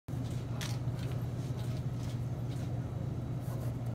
Sheriff McFadden Ends MCSO's Involvement In 287g
Sheriff McFadden signing paperwork to end the Mecklenburg County Sheriff's inolvement with the 287g program.